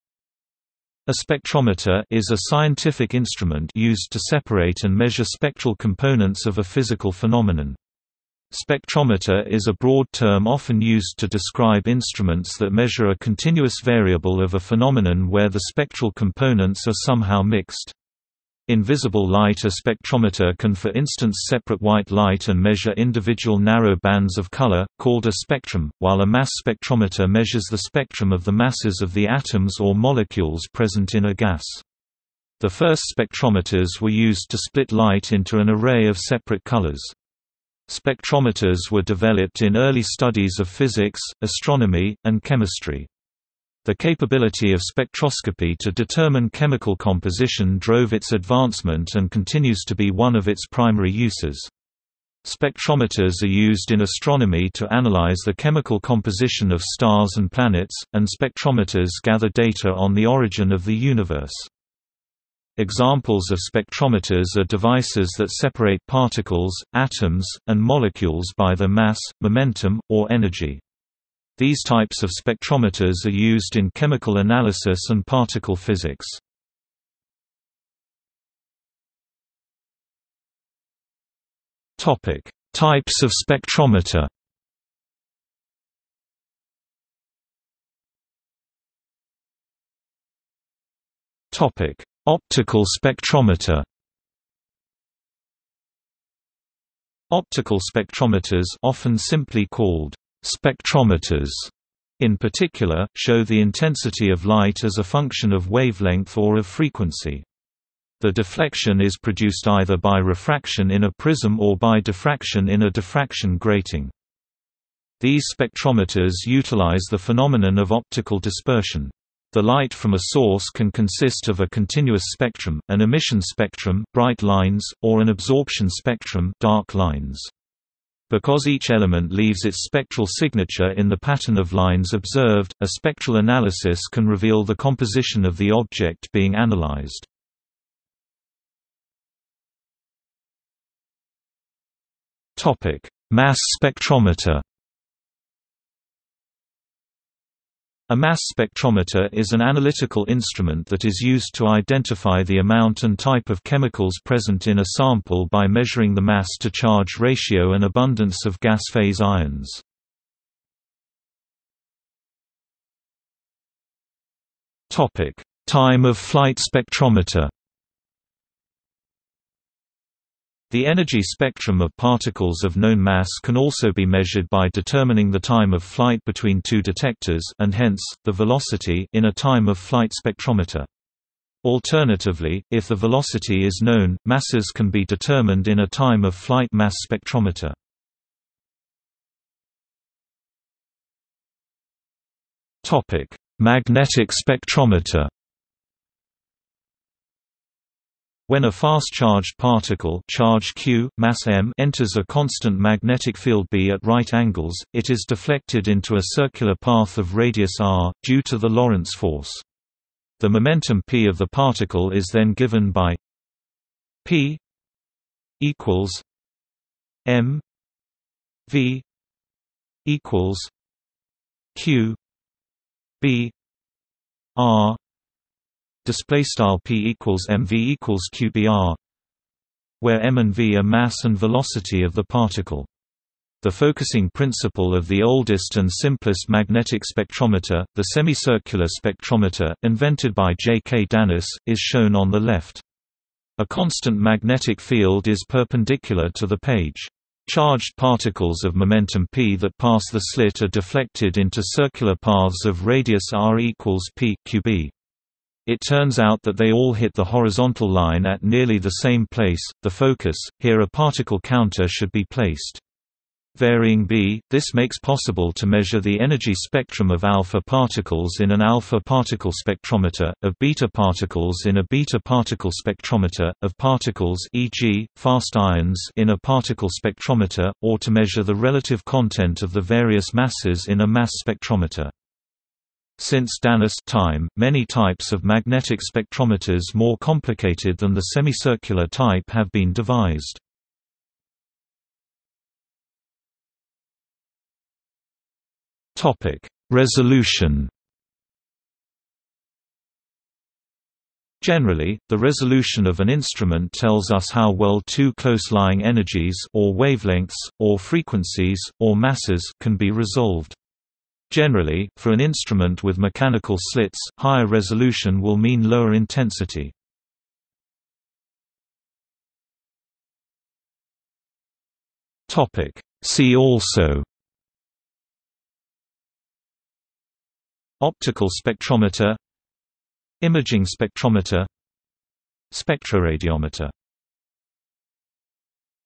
Spectrometer | Wikipedia audio article
00:01:18 1 Types of spectrometer
00:01:28 1.1 Optical spectrometer
00:02:19 1.2 Mass spectrometer
00:02:41 1.2.1 Time-of-flight spectrometer
00:03:09 1.2.2 Magnetic spectrometer
00:04:52 2 Resolution
00:05:18 3 See also

- Socrates

SUMMARY
A spectrometer () is a scientific instrument used to separate and measure spectral components of a physical phenomenon. Spectrometer is a broad term often used to describe instruments that measure a continuous variable of a phenomenon where the spectral components are somehow mixed.  In visible light a spectrometer can for instance separate white light and measure individual narrow bands of color, called a spectrum, while a mass spectrometer measures the spectrum of the masses of the atoms or molecules present in a gas. The first spectrometers were used to split light into an array of separate colors. Spectrometers were developed in early studies of physics, astronomy, and chemistry. The capability of spectroscopy to determine chemical composition drove its advancement and continues to be one of its primary uses. Spectrometers are used in astronomy to analyze the chemical composition of stars and planets, and spectrometers gather data on the origin of the universe.
Examples of spectrometers are devices that separate particles, atoms, and molecules by their mass, momentum, or energy. These types of spectrometers are used in chemical analysis and particle physics.